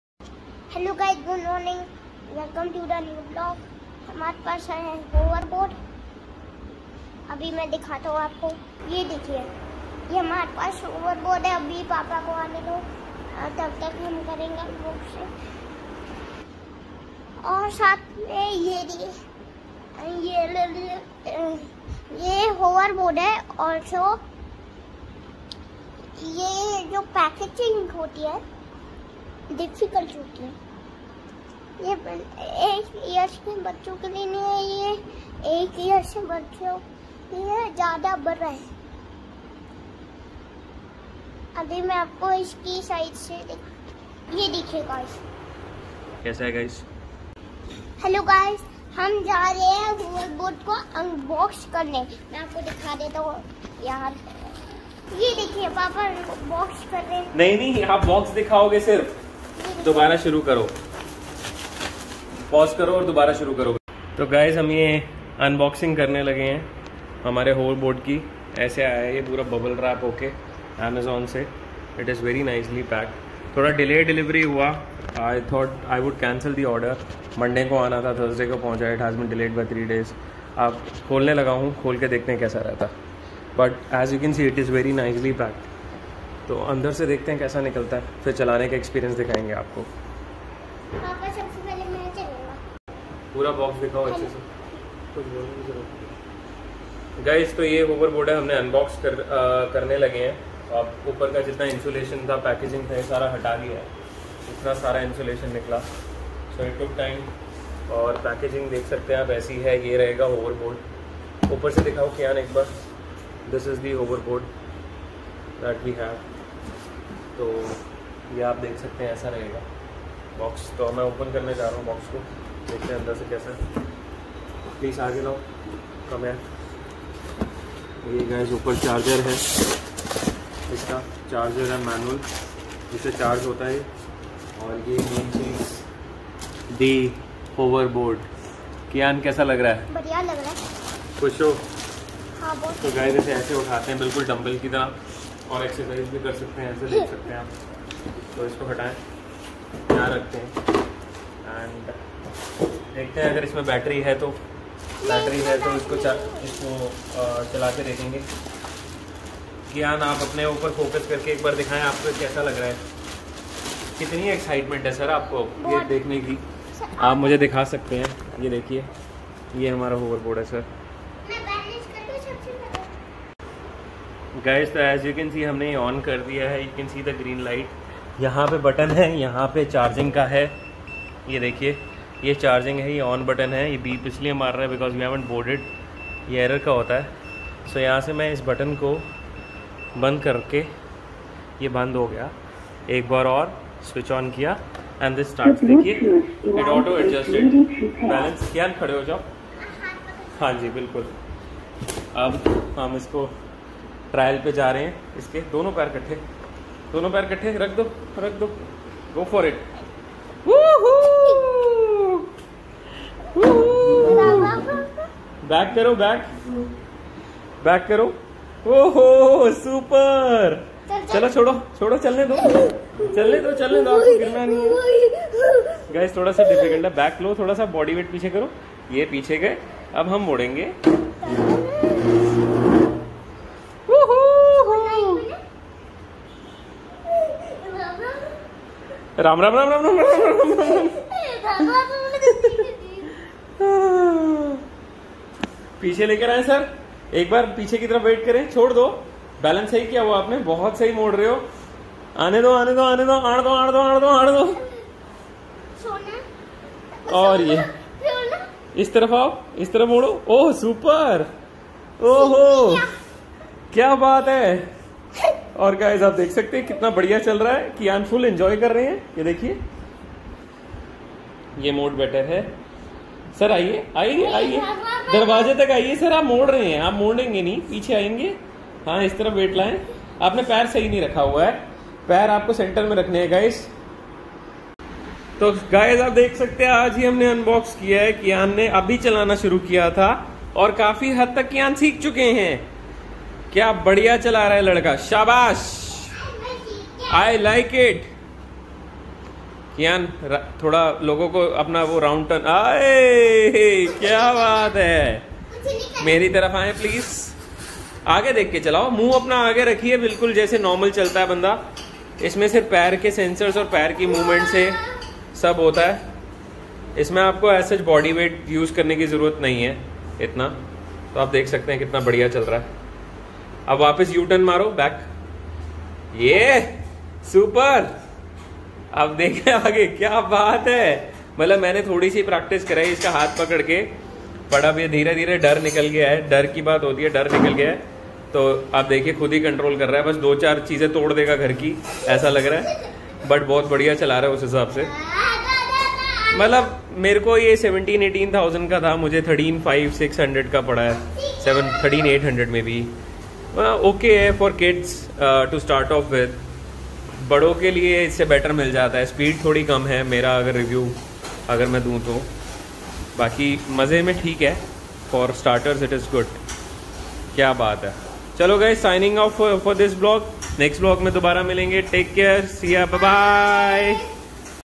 Kiaan got a new hoverboard | would he be able to ride it?😎
Kiaan riding his new Hoverboard...Trust me he rode it like a professional...

Must Try for Kids above 6 yrs of age..